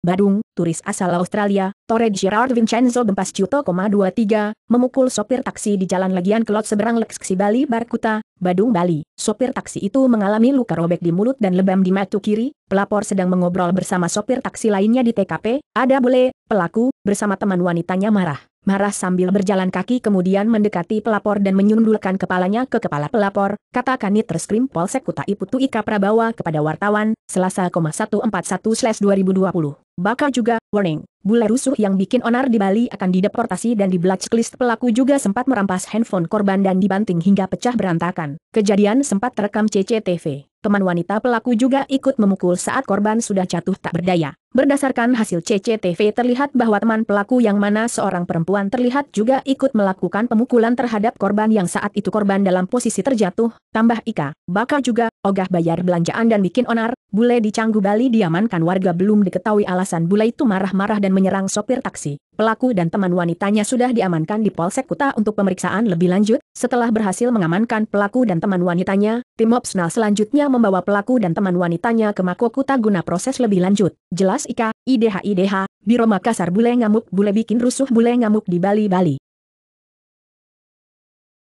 Bule Australia Aniaya Sopir Taksi hingga Mulut Robek-Mata Lebam di Bali
Badung - Turis asal Australia, Tore Gerard Vincenzo Bempasciuto ,23, memukul sopir taksi di Jalan Legian Kelod seberang Lexxy Bali Bar Kuta, Badung Bali. Sopir taksi itu mengalami luka robek di mulut dan lebam di mata kiri. ,Pelapor sedang mengobrol bersama sopir taksi lainnya di TKP, ada bule ,pela